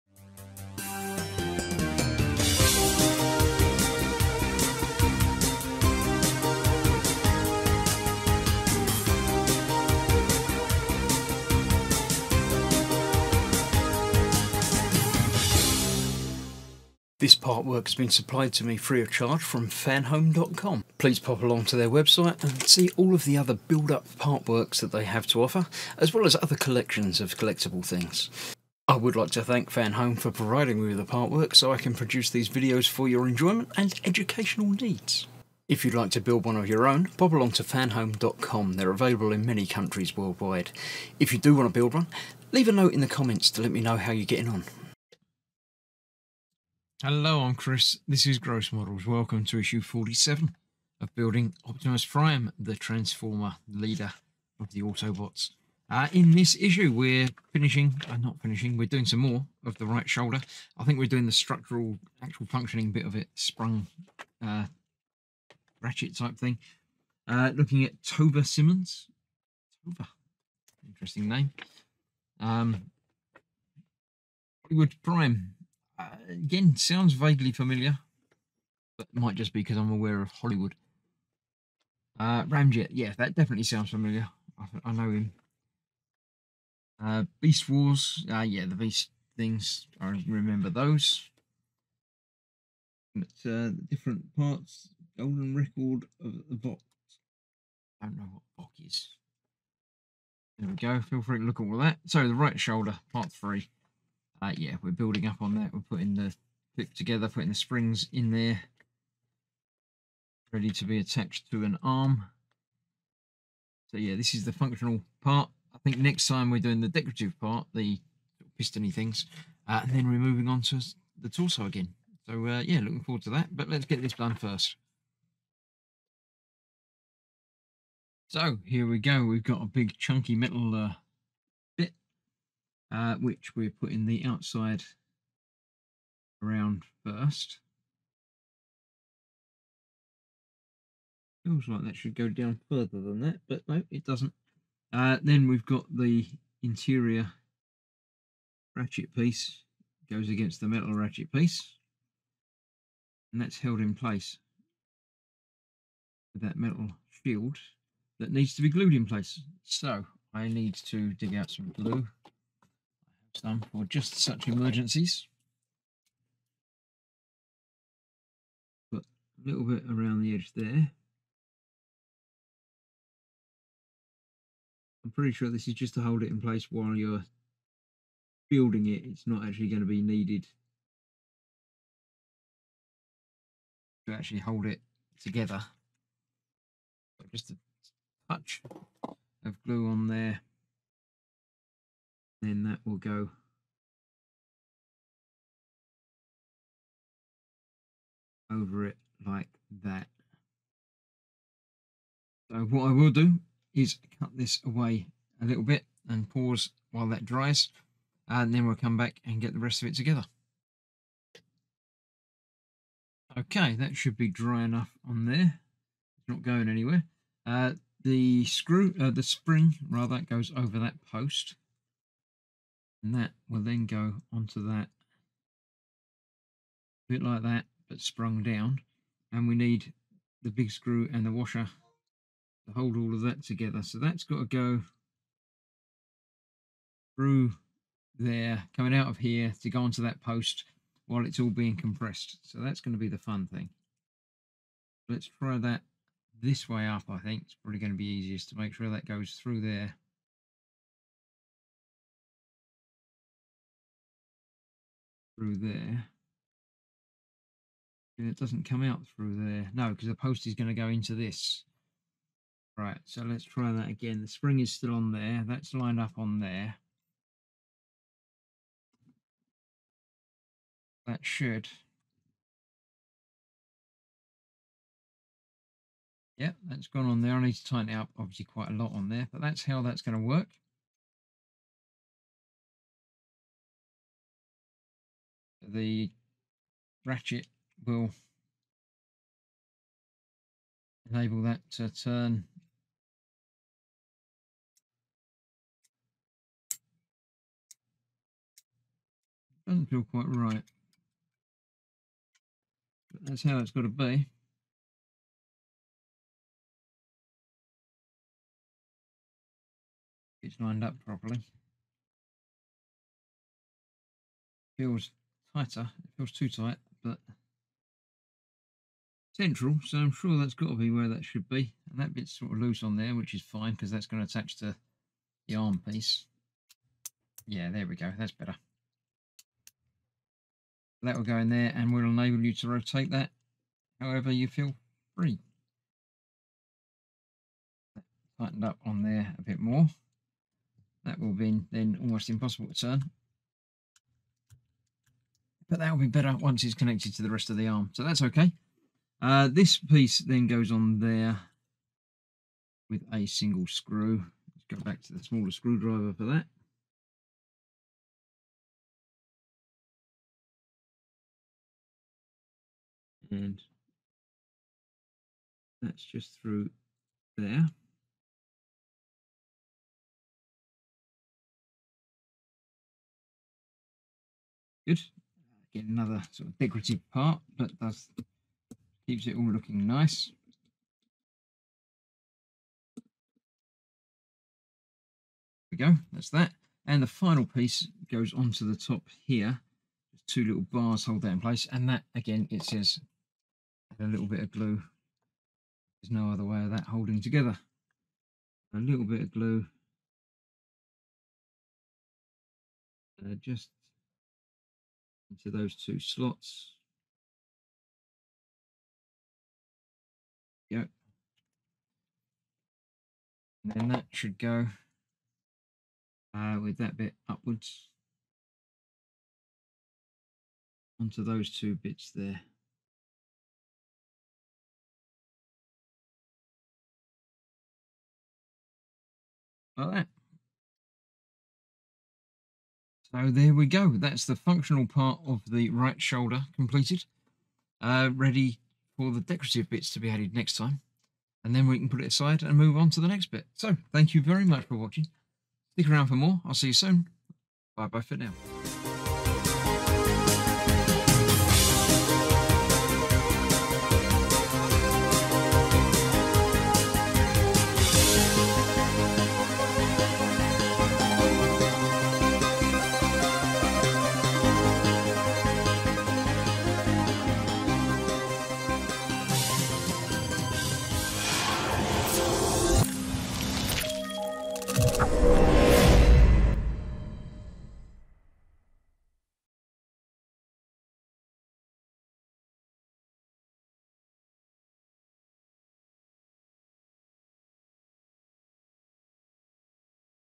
Fanhome Optumus Prime build. Issue 47
Welcome to another issue of the Optimus Prime build from Transformers,, kindly supplied to me by Fanhome.  If you would like to build your own,

In this issue I will be continuing the right shoulder.

Shop for your own wooden kits!

Join the Modelmakers Boomhut to see lots of amazing creations from around the world and become part of a great community!